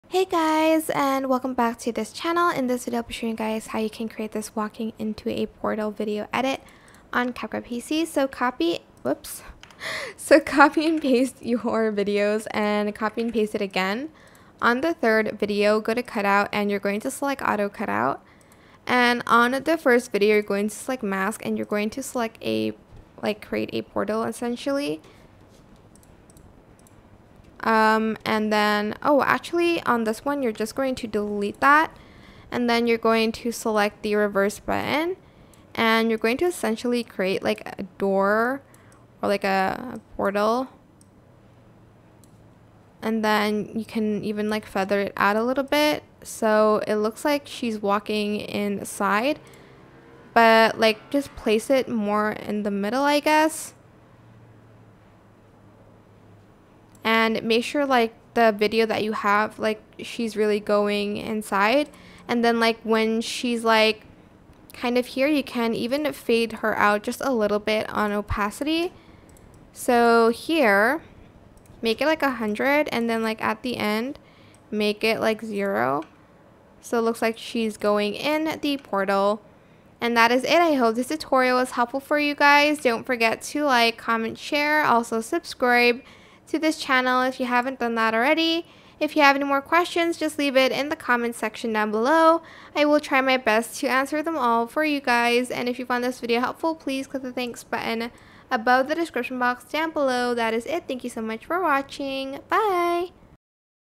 ~ Want to Add Magic to Your Videos? Learn How to Create a Portal Effect on CapCut PC Now!
Step into the realm of magic and wonder with our latest tutorial on how to create a mesmerizing portal effect using CapCut PC! Are you ready to elevate your video editing game and captivate your audience like never before? In this step-by-step guide, we'll walk you through the process of adding a touch of enchantment to your videos with just a few simple clicks.

Creating a portal effect has never been easier, thanks to the powerful tools available in CapCut PC. Whether you're a beginner or an experienced editor, you'll find our tutorial easy to follow and highly effective in achieving stunning results.

First, we'll guide you through the process of duplicating your video and using the auto cutout feature to create the illusion of a portal. Then, we'll show you how to refine the effect by adjusting the mask and feather settings to make it look seamless and magical.

But that's not all – we'll also share some pro tips on enhancing the realism of your portal effect, such as adjusting opacity and adding subtle animations to bring your video to life.

So why wait? Join us on this magical journey and transform your videos into captivating works of art!

Ready to add a touch of magic to your videos? Let's dive in and create something truly extraordinary together!